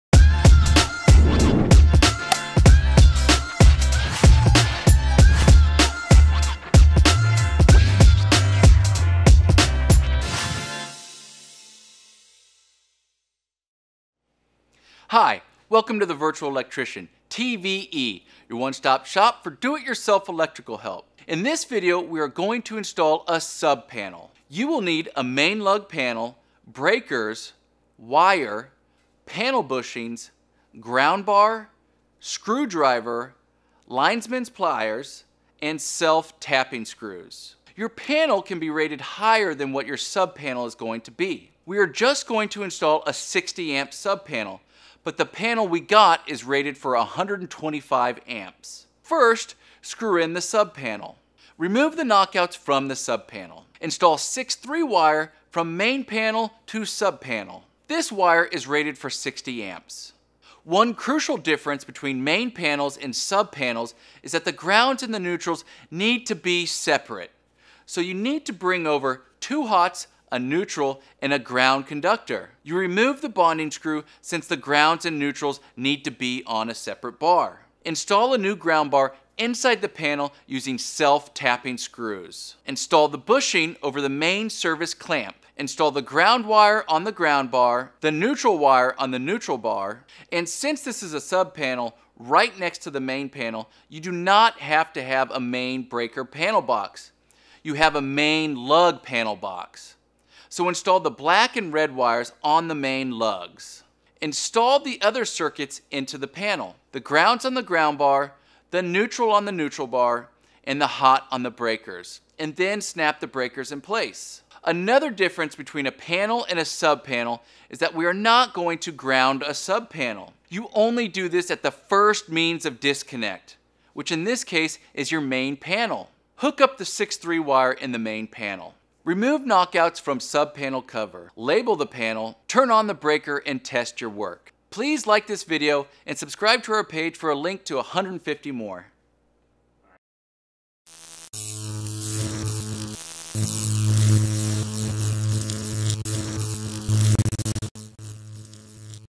Installing a Subpanel
Very quick video about installing a Subpanel. This is more of a refresher video for actual electricians.  If you have never installed an electrical subpanel before. Hop on the comment section and I can give you more detailed instructions or head on over